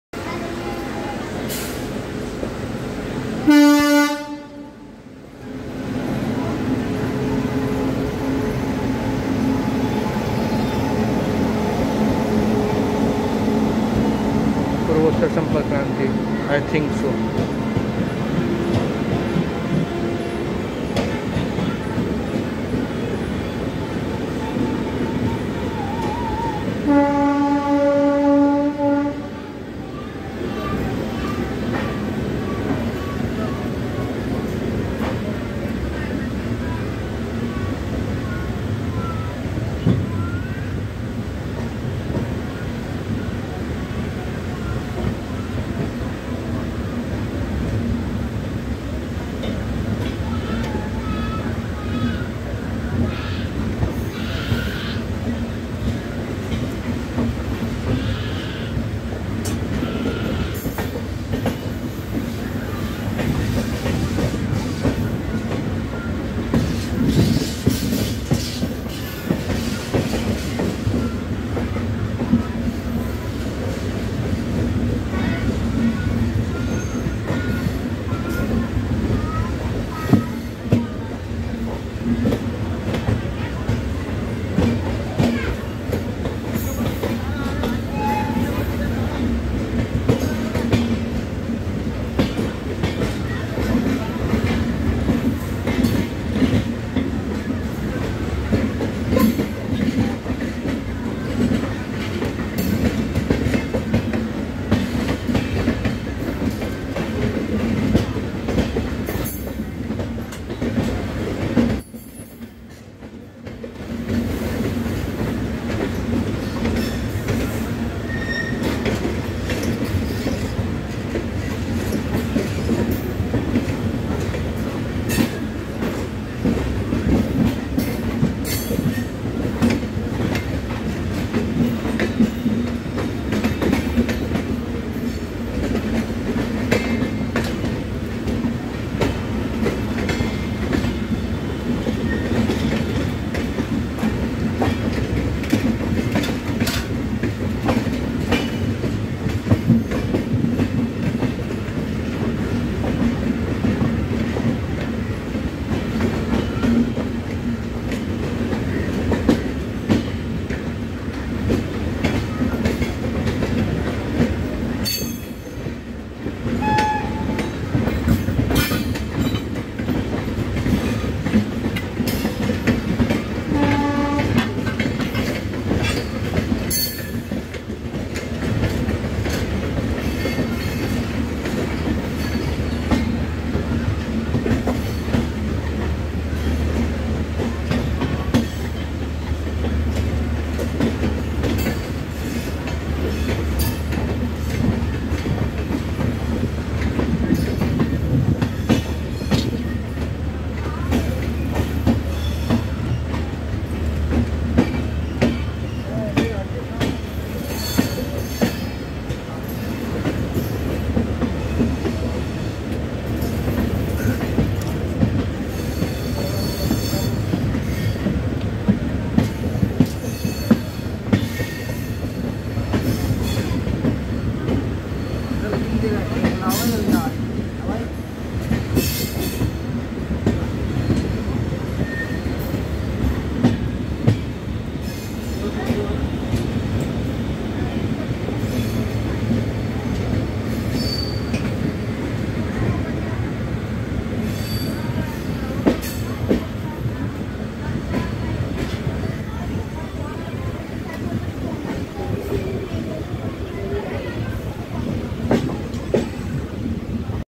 Parallel Arrival at New Delhi
onboard : JUC NDLS ICE
feat. train : Poorvotar S Kranti Exp (GHY-NDLS)